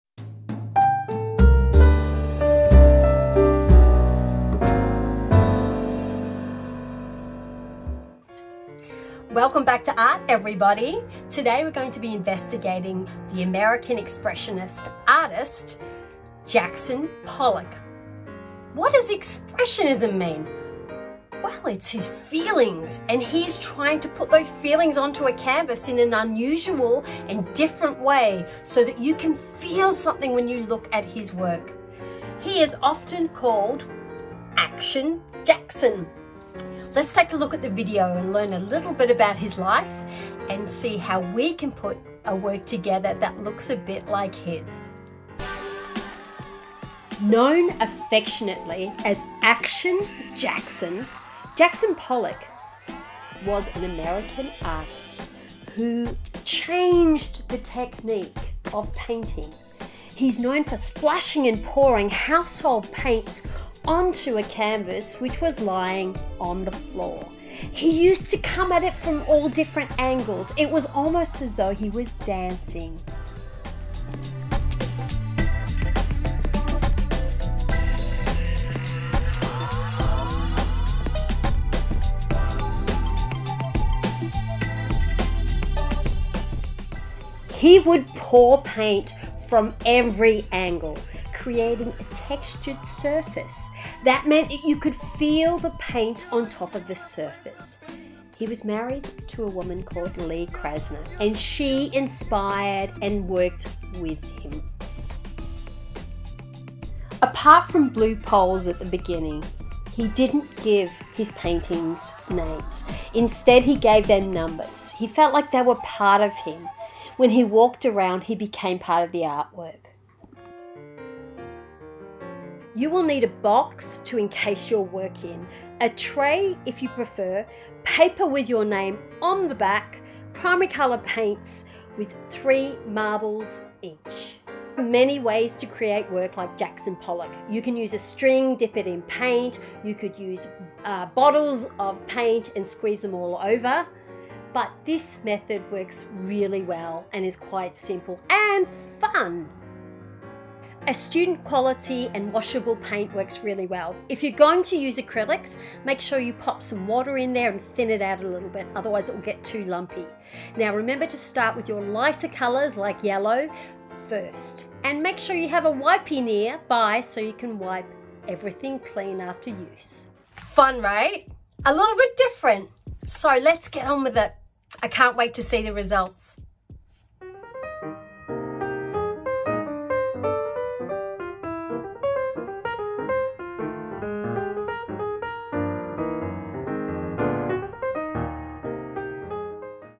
Action Jackson
How to create a Jackson Pollock abstract painting.